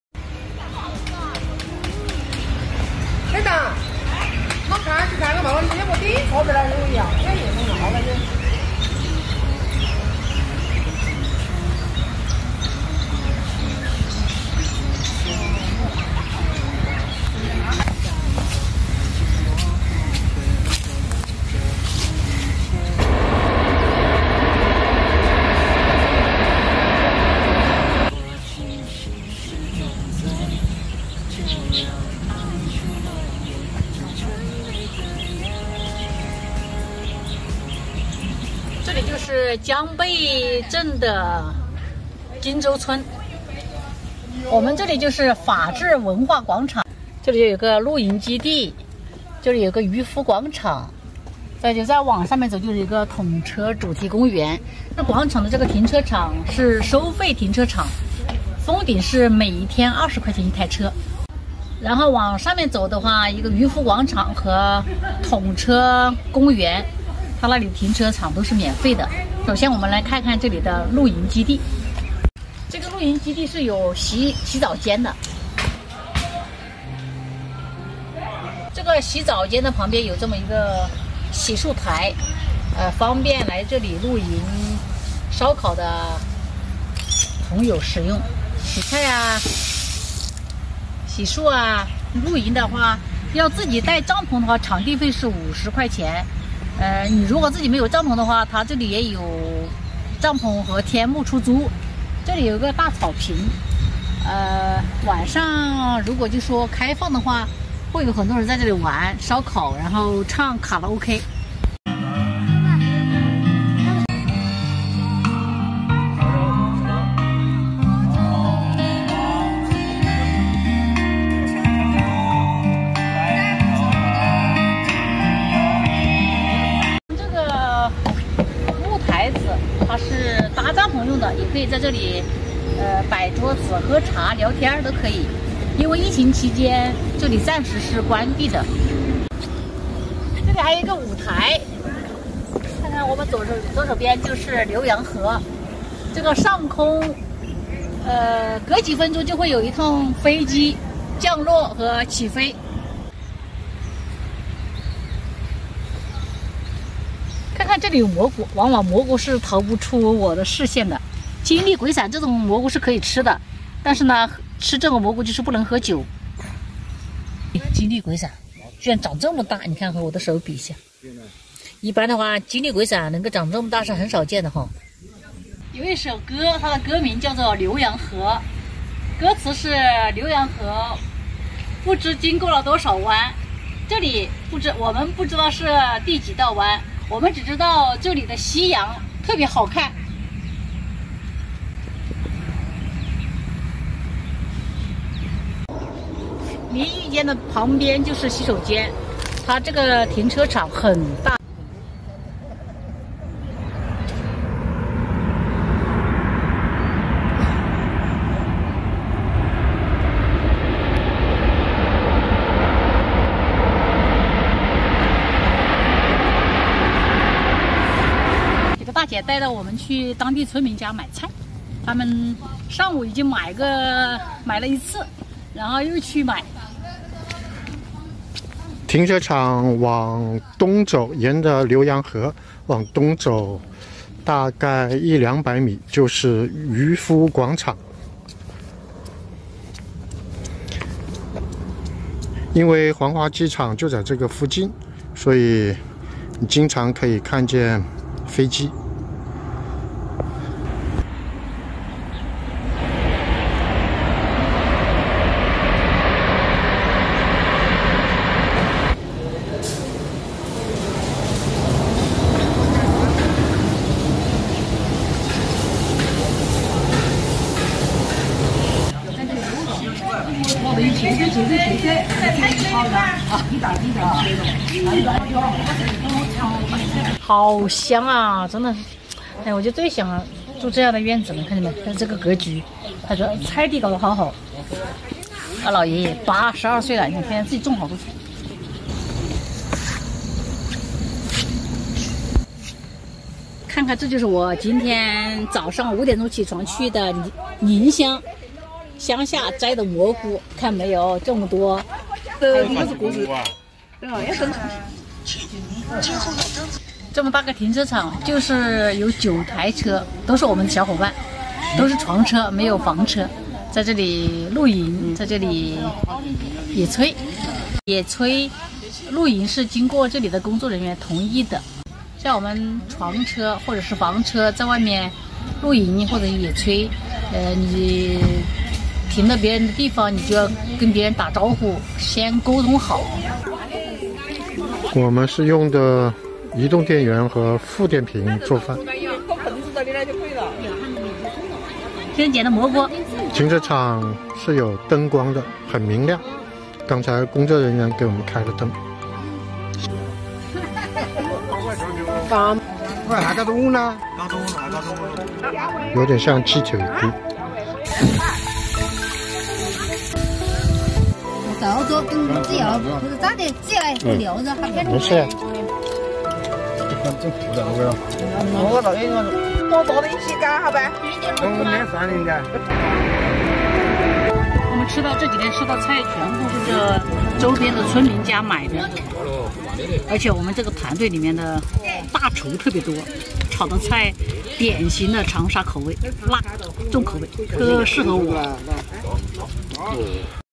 9台床车长沙周边游，金洲村是个好地方，浏阳河畔露营基地夕阳美
6070旅居自驾游
优质vlog领域创作者
家有肥猫，其主好玩。一车两人，四海为家。